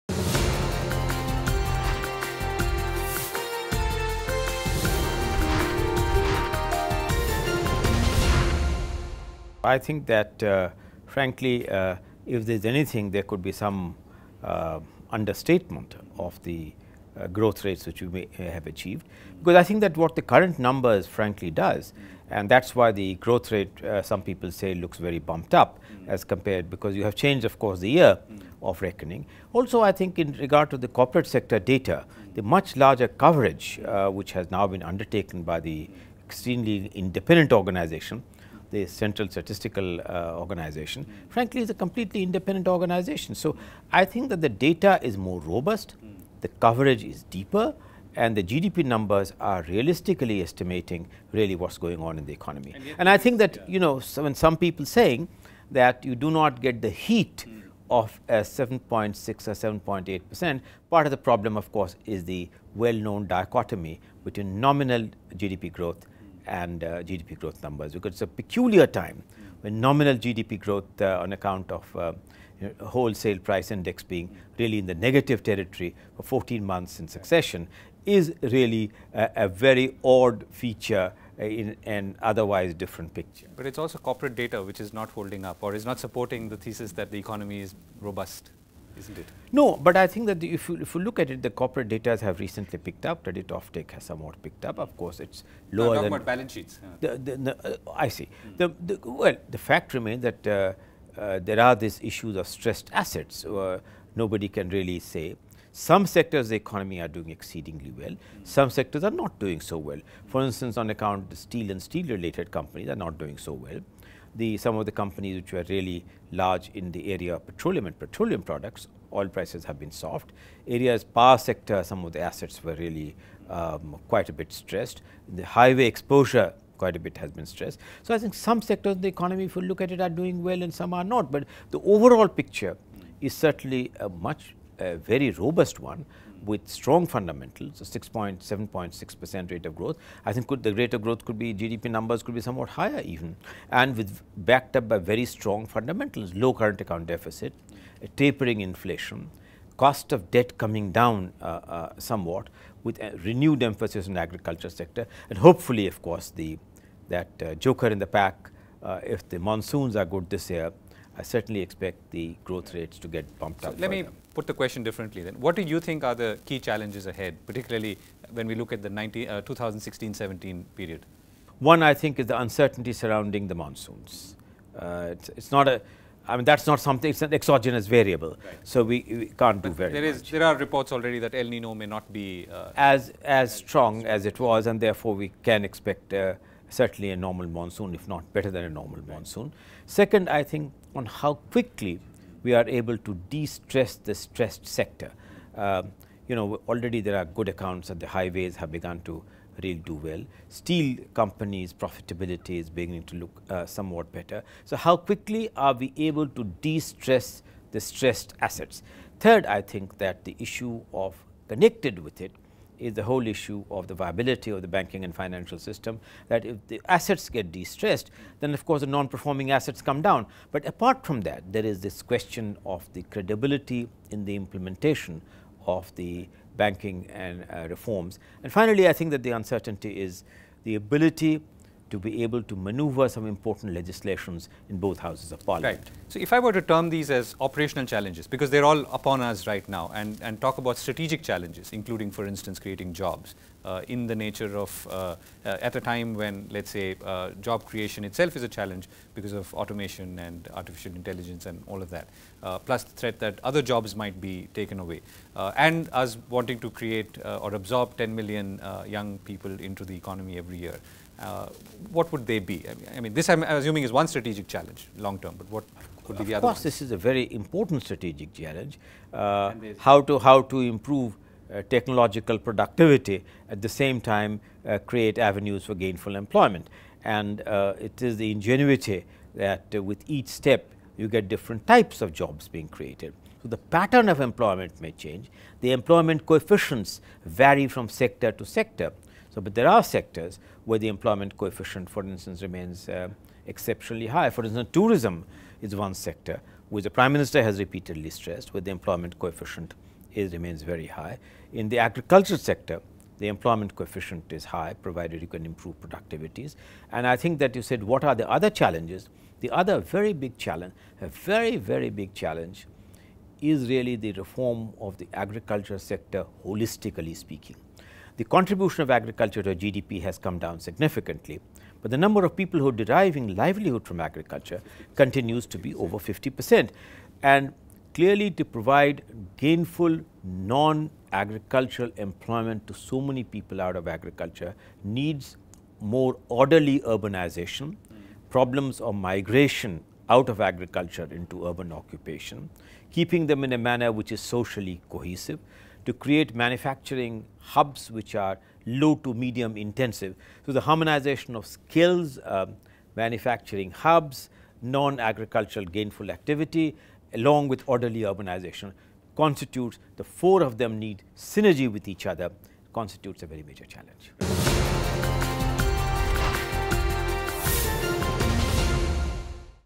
N K Singh || The Growth Net Summit 4.0
N K Singh, Chairman, Steering Committee, The Growth Net Summit, in conversation with Govindraj Ethiraj, Ping Network, at The Growth Net Summit 4.0